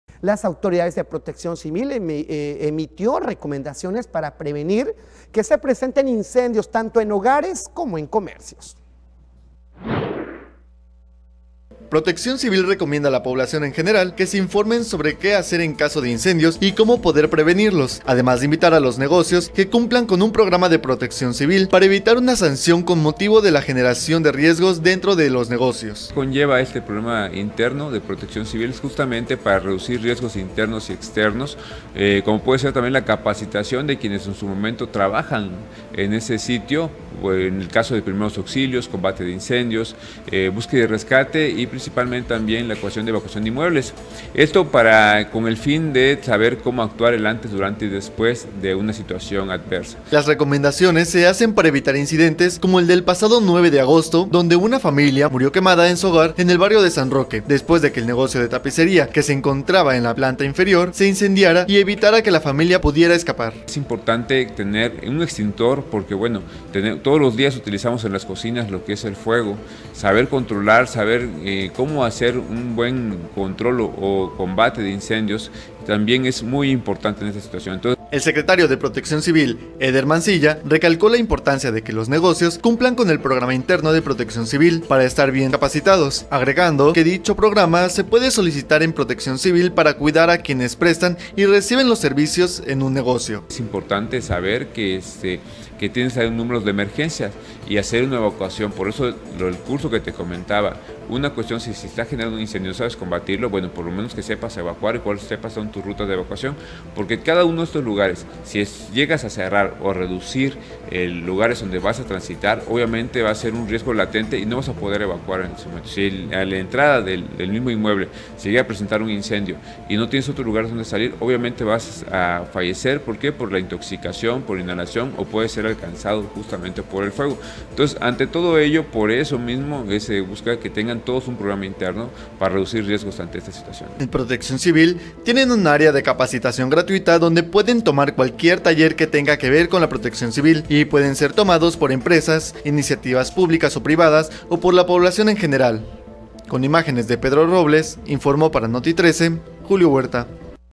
Protección civil emite recomendaciones para prevenir incendios en los hogares | NOTITRECE
🔻ImportanteSaber | El Secretario de ProtecciónCivil, Eder Mancilla, recalcó la importancia de que los negocios cumplan con el programa interno de protección civil para estar bien capacitados, agregando que dicho programa se puede solicitar en protección civil.

📝Julio Huerta
Síguenos también, en nuestra página oficial: